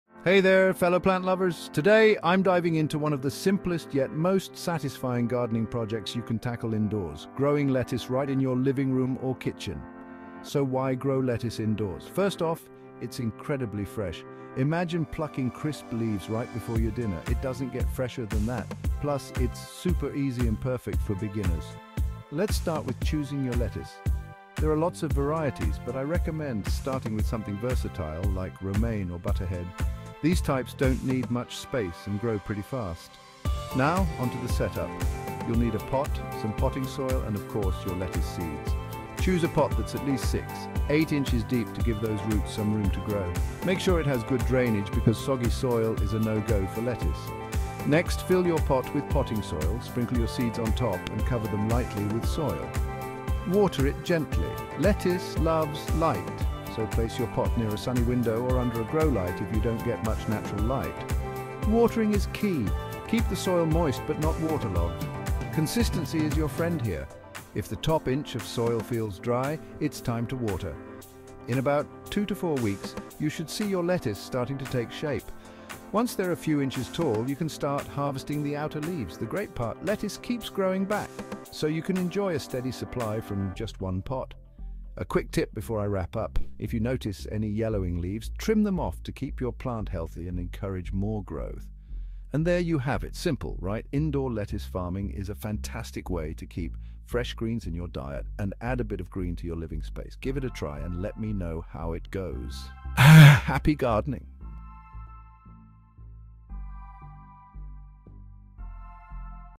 Grow Lettuce Indoors: Easy Pot Gardening!
In this video, we will share the easiest indoor garden lettuce growing hacks for those who don't have outdoor space. You will learn how to successfully grow lettuce in pots indoors, allowing you to enjoy fresh and homegrown greens all year round. Watch till the end for valuable tips and tricks to ensure a bountiful harvest of delicious lettuce!